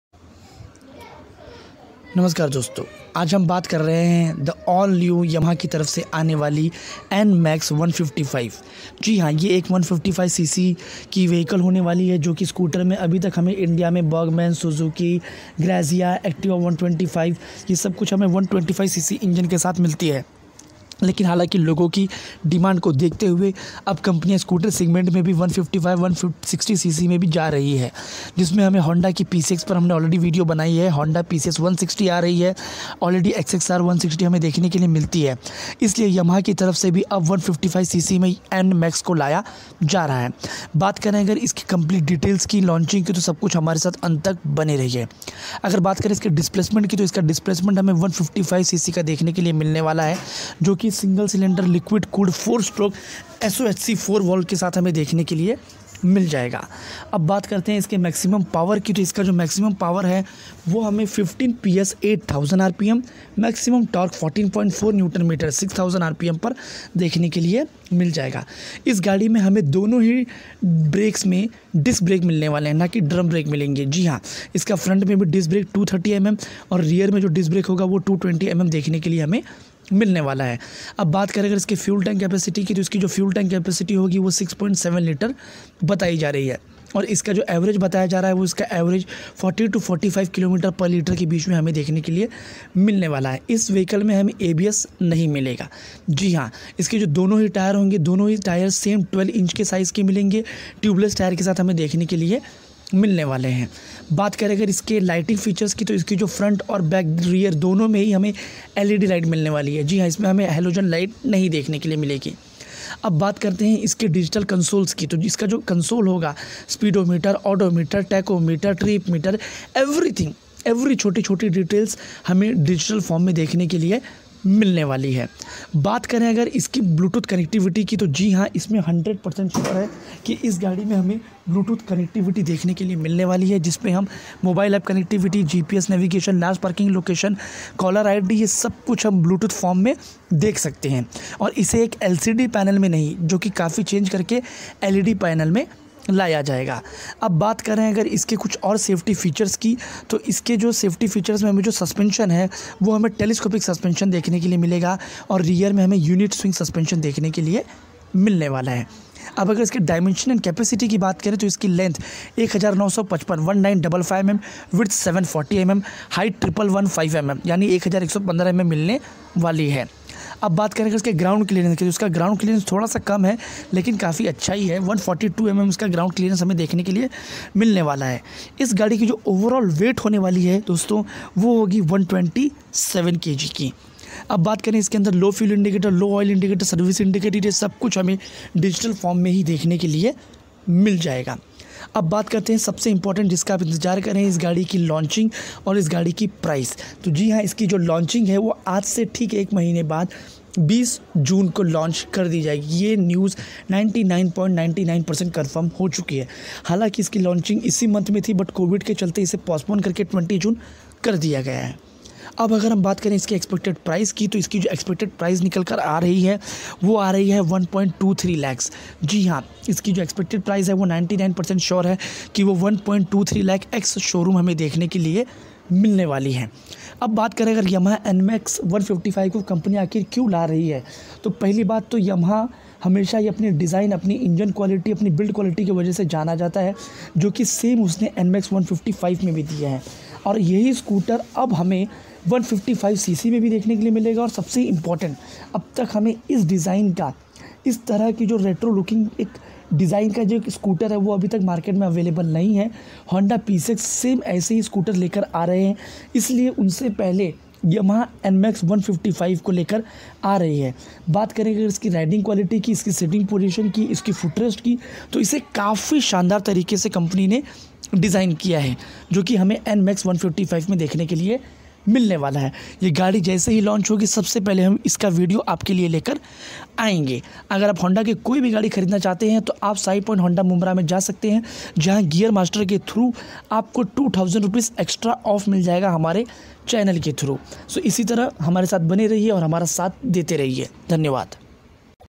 All new Upcoming Yamaha NMAX 155 | Detailed review | Price | Confirmed Launch date!?
Yamaha Nmax 155 is going to be launched in India, we have given a detailed review about its specifications, features, price and its launch date.